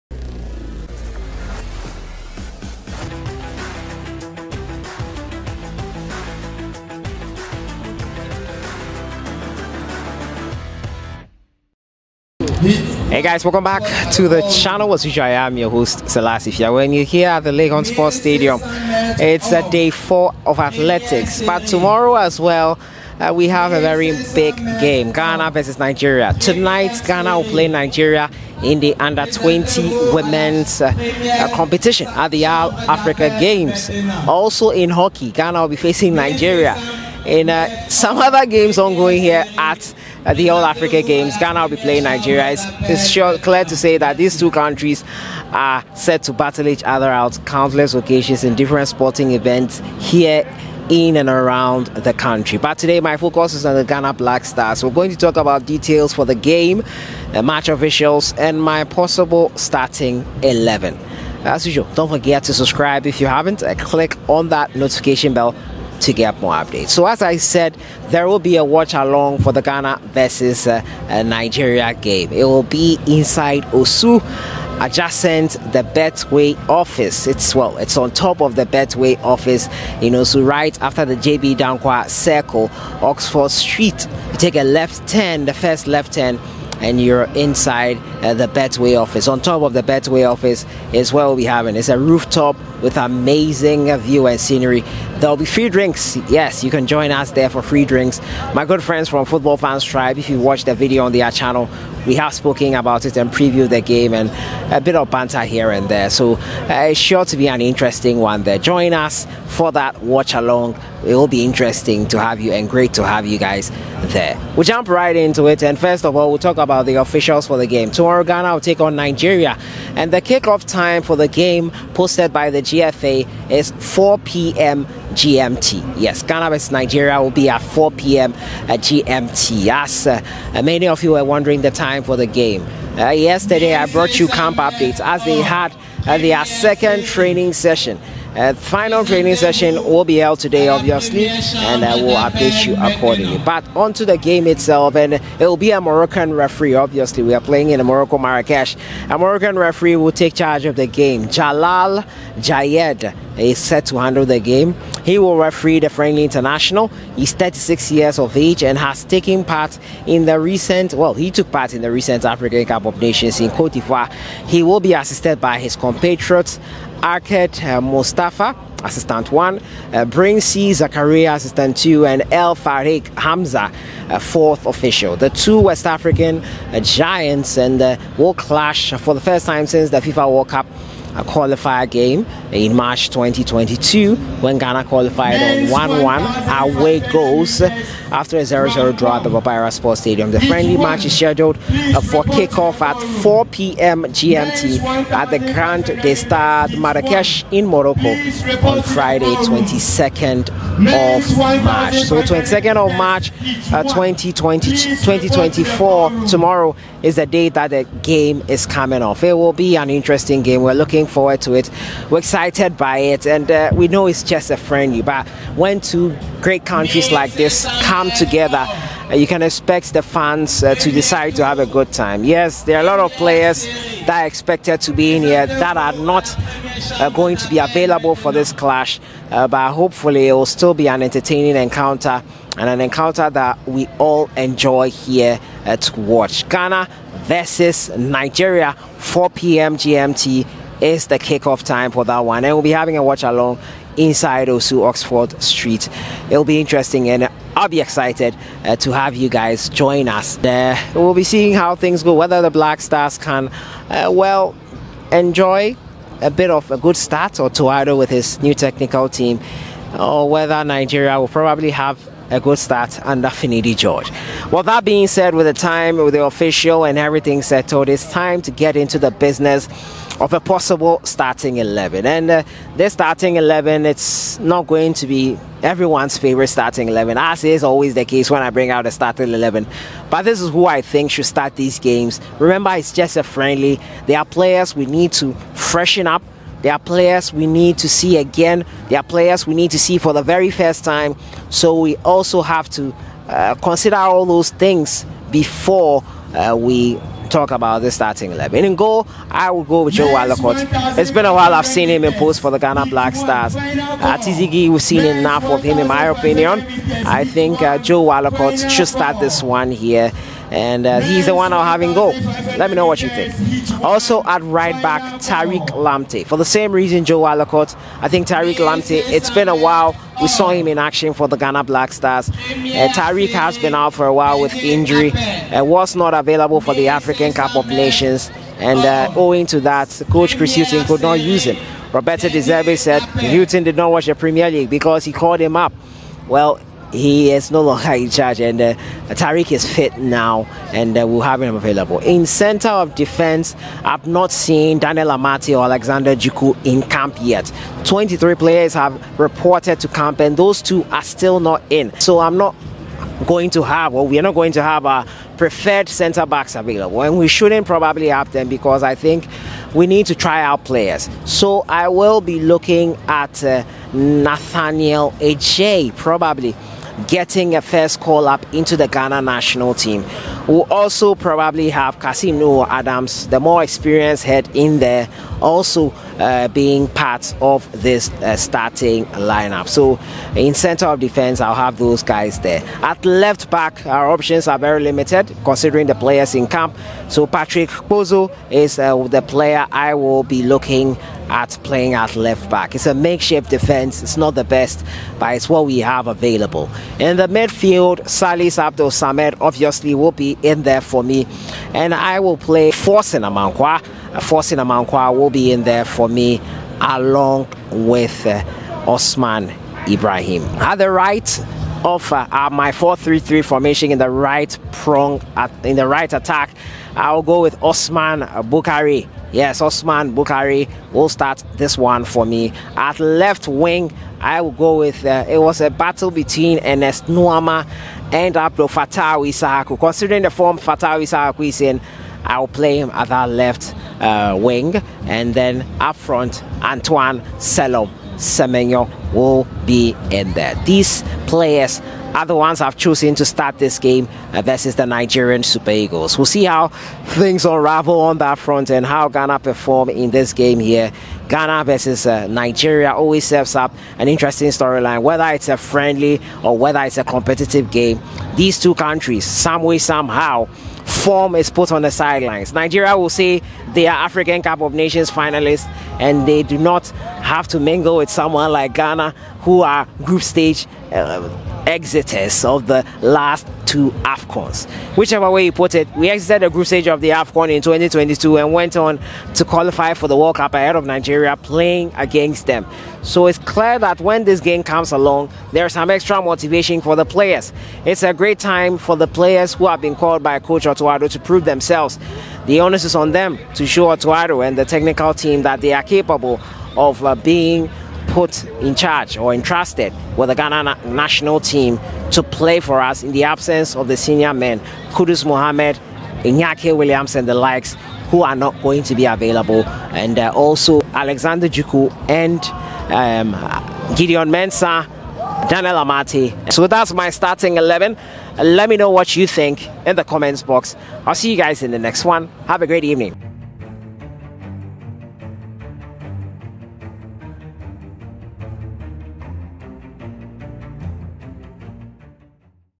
GHANA VS NIGERIA-BLACK STARS POSSIBLE STARTING XI TO FACE SUPER EAGLES
WE HAVE A LOOK AT THE GAME BETWEEN GHANA AND NIGERIA AND THE POSSIBLE STARTING XI TO FACE THE NIGERIANS